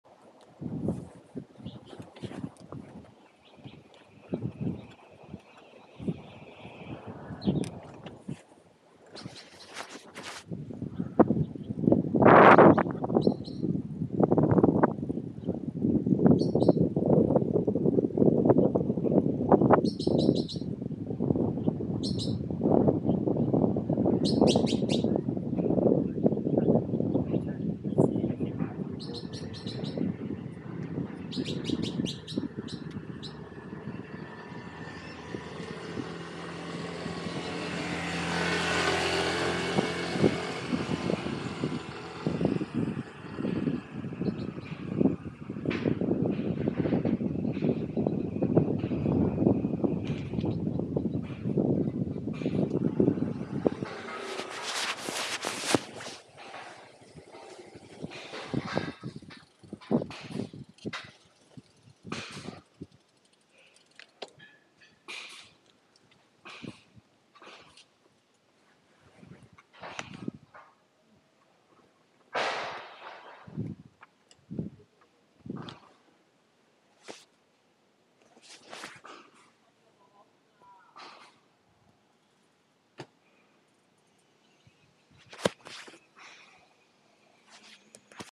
單車紀錄：承天禪寺
今天高溫20度，出太陽藍天白雲好天氣。可以看到大屯山系及觀音山在同一視野。